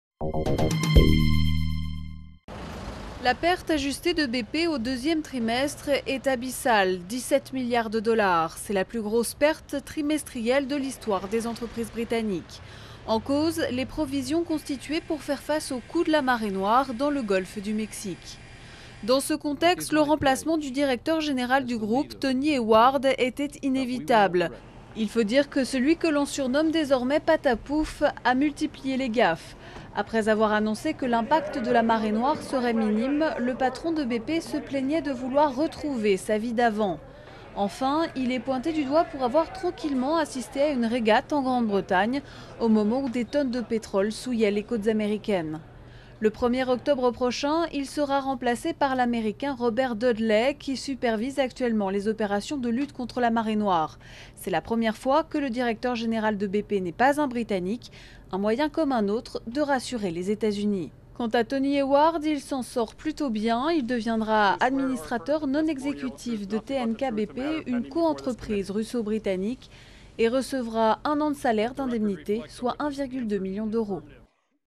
BP : une perte colossale et un nouveau patron
Trois mois après le début de la plus grande catastrophe écologique de l'histoire des Etats-Unis, BP affiche une lourde perte au second trimestre. Le patron du groupe pétrolier, Tony Hayward, annonce sa démission prochaine. Durée: 01mn15sec